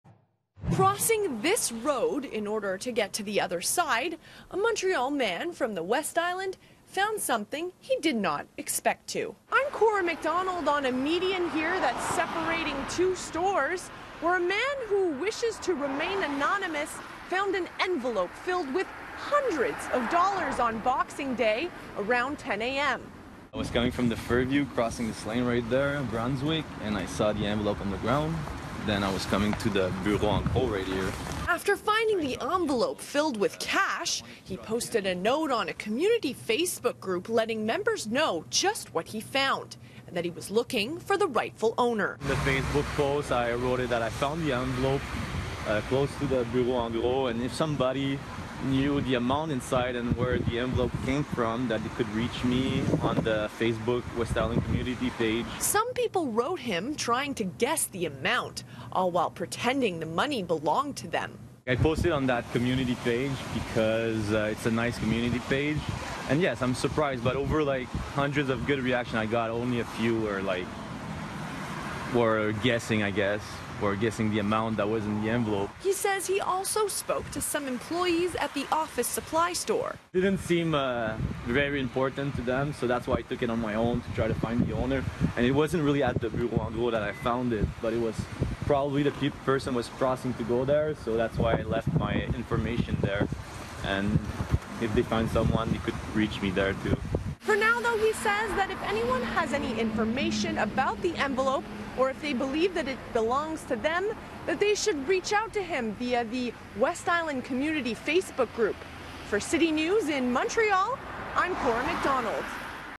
Montreal man using social media to find owner of cash found on street
A Montreal man is using social media to try to return an envelope of cash he found while crossing the street.